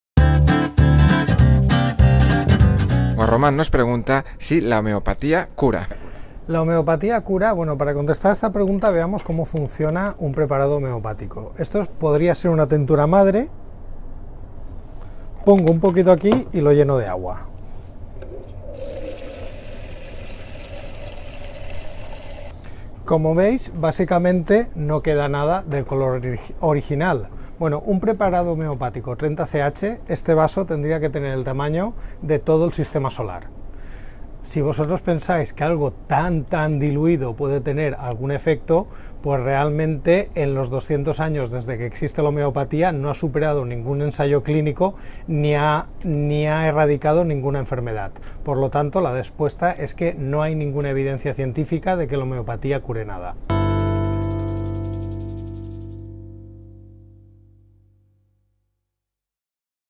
UPVdivulga #06 - Homeopatía - José Miguel Mulet
José Miguel Mulet responde a una pregunta sobre la eficacia de la homeopatía.